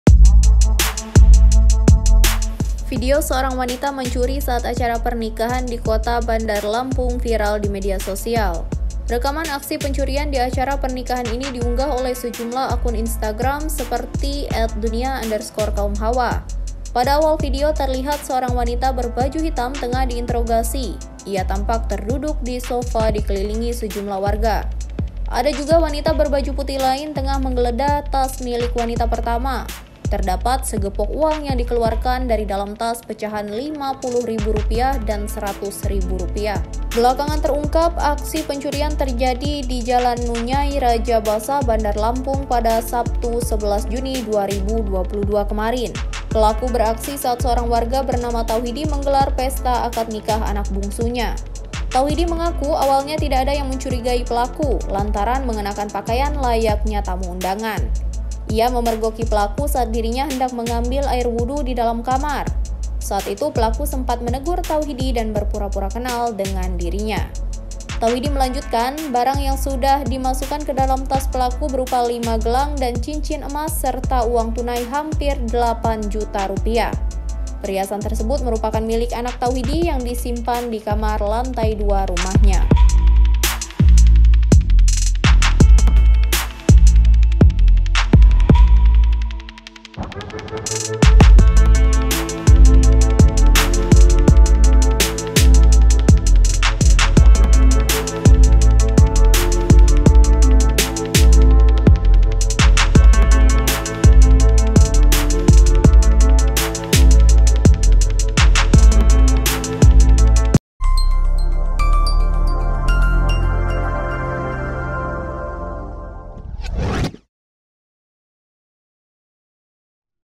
Viral Wanita Mencuri saat Acara Pernikahan, Nyamar Jadi Tamu Undangan, Gasak Emas dan Segepok Uang
Rekaman aksi pencurian di acara pernikahan ini diunggah oleh sejumlah akun Instagram, seperti dunia_kaumhawa.

Pada awal video terlihat seorang wanita berbaju hitam tengah diinterogasi.

Ia tampak terduduk di sofa dikelilingi sejumlah warga.

Ada juga wanita berbaju putih lain tengah menggeledah tas milik wanita pertama.

Ada segepok uang yang dikeluarkan dari dalam tas pecahan Rp 50 ribu dan Rp 100 ribu.